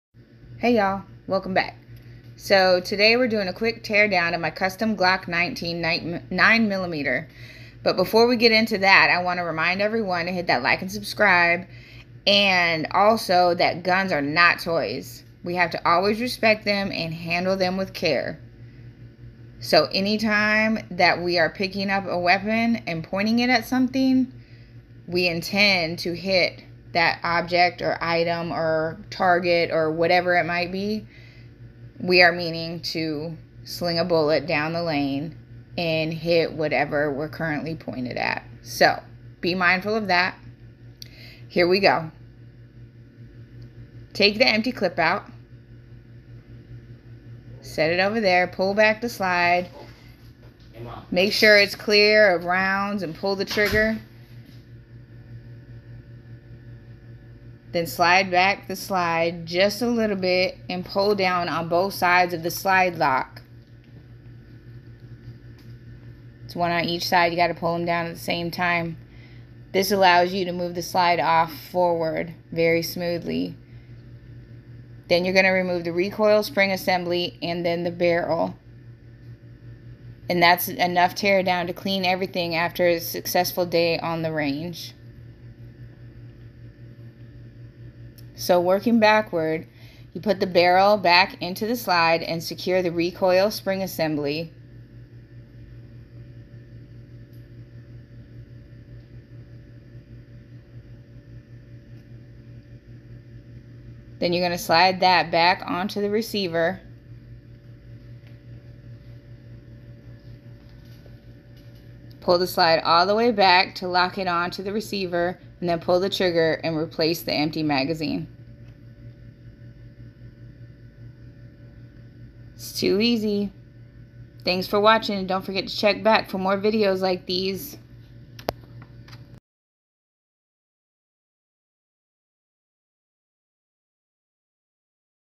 Glock 19 9MM basic tear down
My husband bought me this glock for my birthday about 10 years ago,  I absolutely love it!